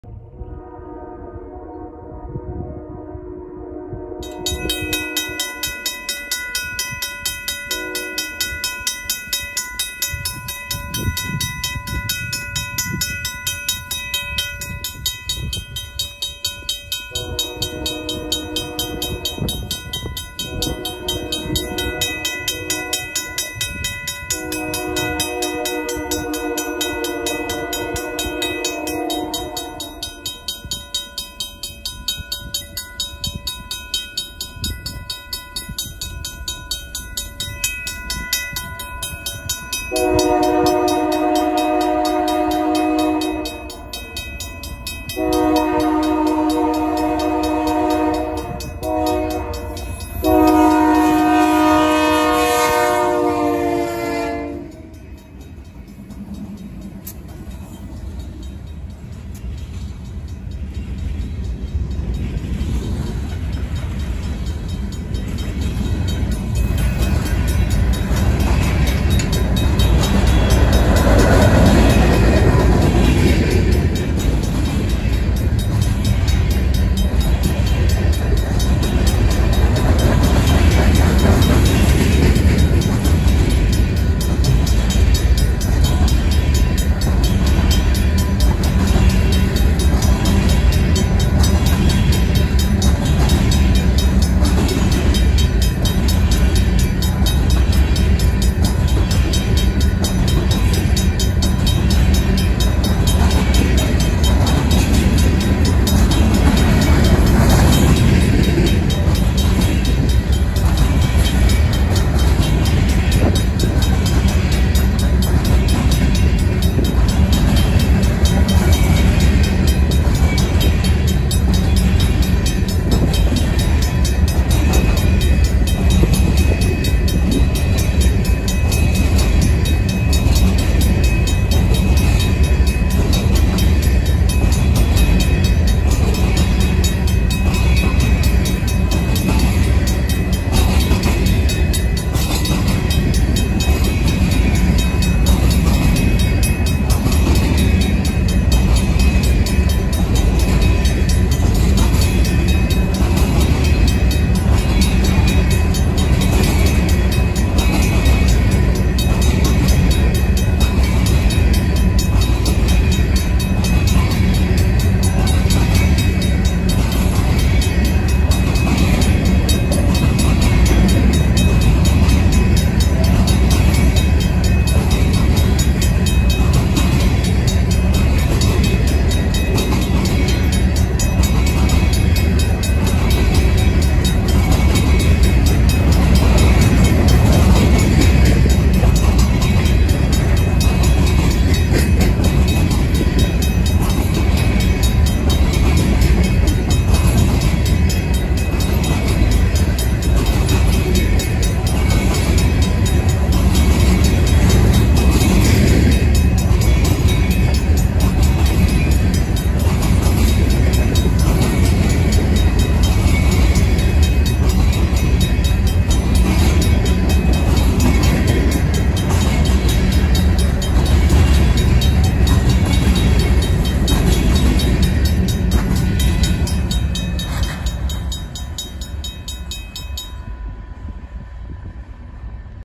Emerald Street railroad crossing, UP 6883 Grain Train, Onawa, IA
A Union Pacific grain train heads north through Onawa. They ended up stopping at the grain elevator in Sloan.
Crossing info
Owner and maintainer: Union Pacific Railroad
Location: Onawa, Iowa
Line: Union Pacific Sioux City Subdivision
Left signal
Lights: 2 pairs of Safetran 12x24 inch with GE Dotted LEDs
Right signal
Bell: RACO mechanical bell